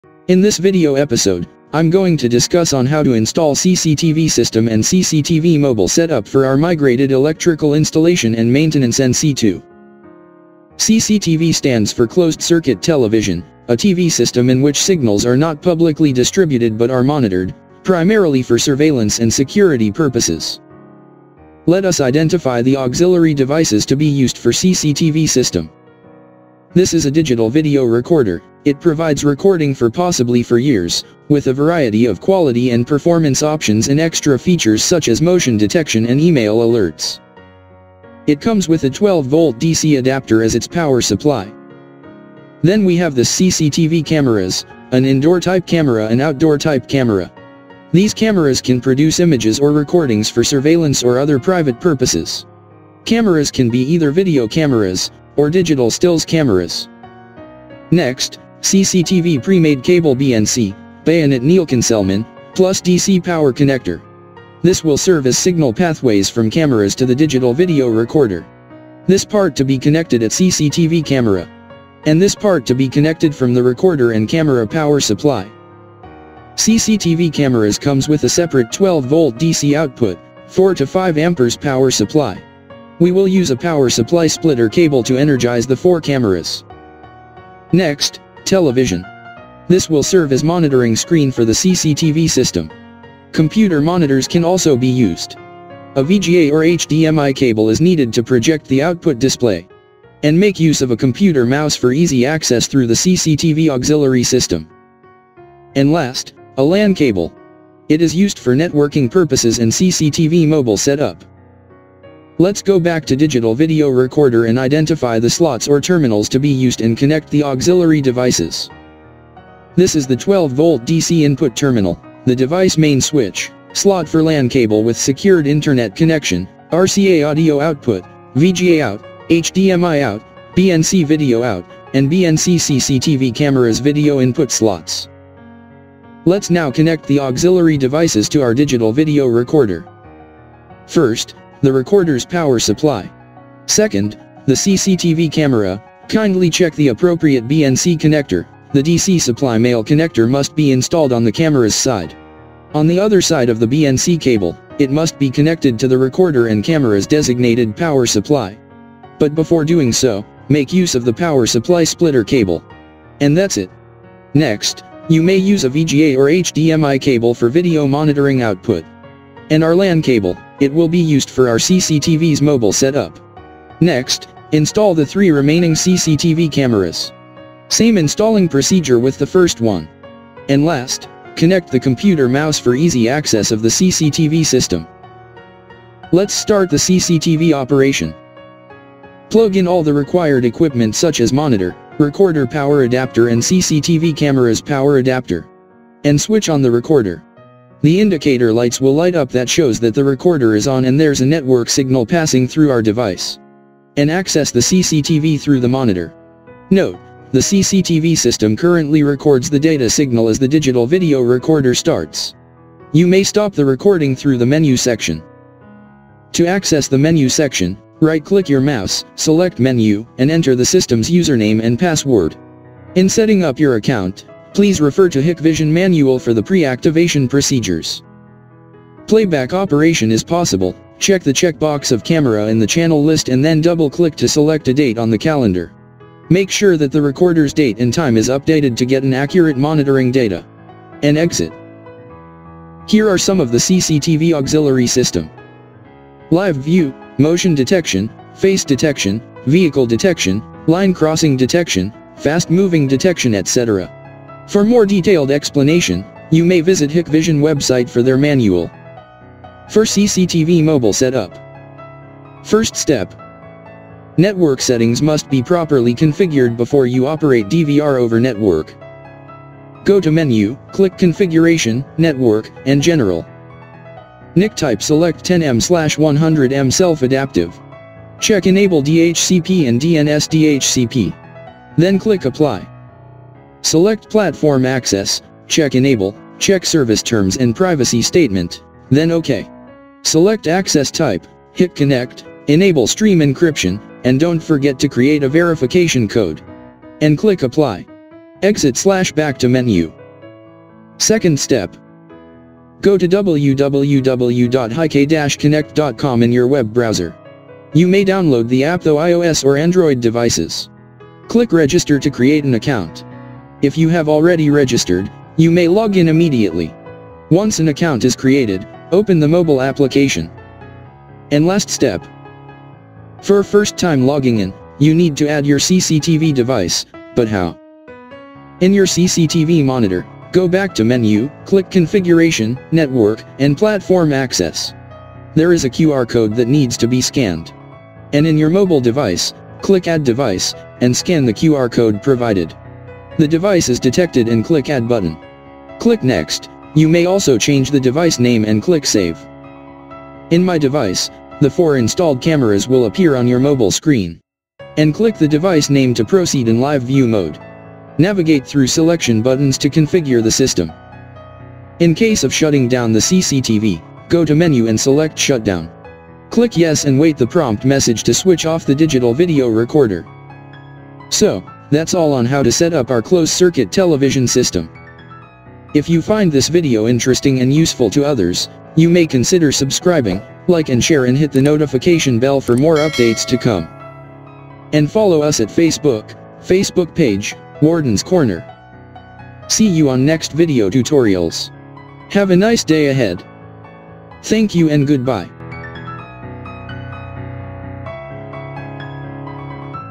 CCTV | Electrical Installation and Maintenance NC II
How to set-up CCTV networking on iOS and Android mobile devices

Content/s:
a) CCTV Definition
b) CCTV Auxiliary Devices
c) CCTV Auxiliary System
d) CCTV Mobile Set-up